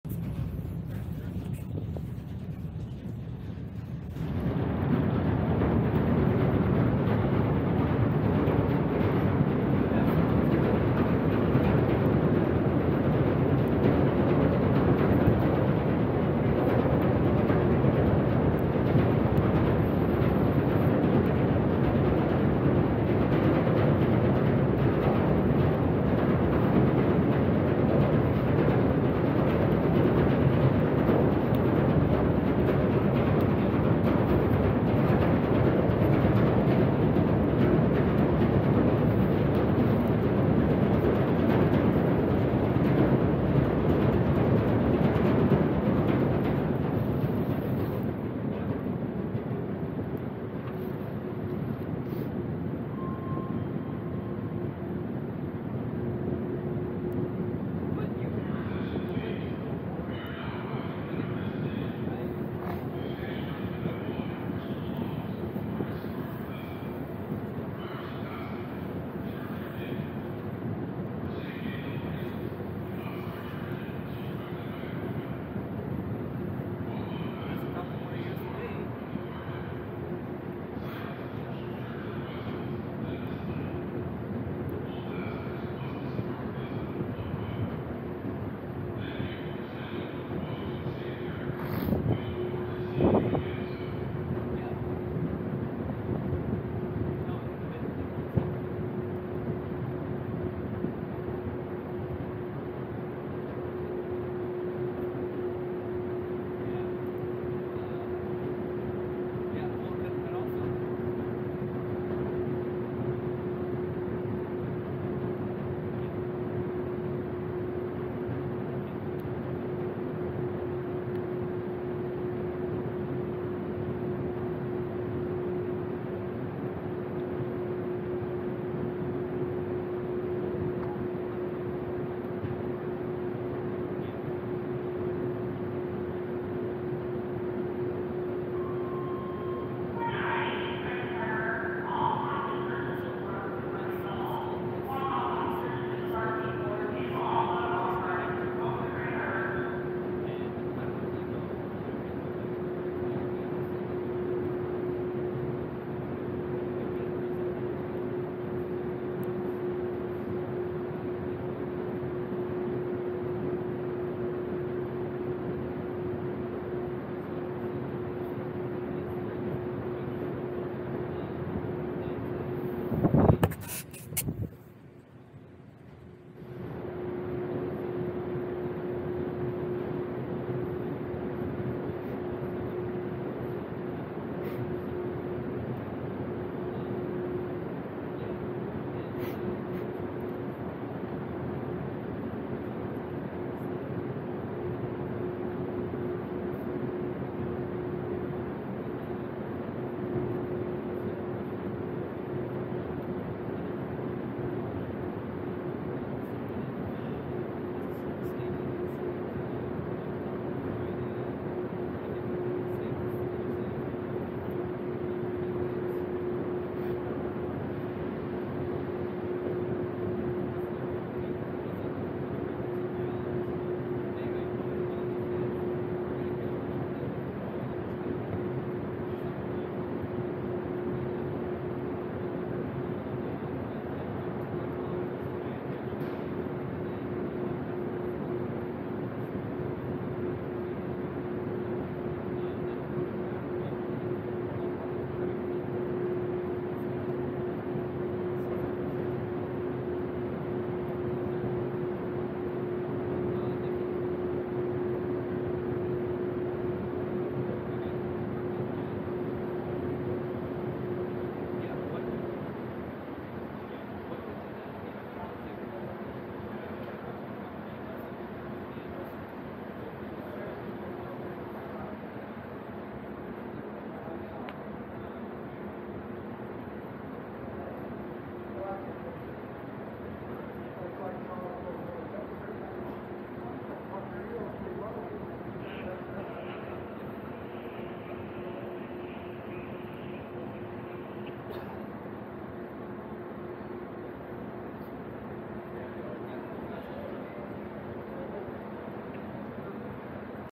Insta360 - Aboard Washington State Ferry, M/V Kaleetan approaching Friday Harbor Terminal
insta360 wsf firstpersonview FridayHarbor My Insta360R video from the passenger deck of the Washington State Ferry, M/V Kaleetan on approach to Friday Harbor, WA Terminal on March 13, 2023 - 8:55am sailing.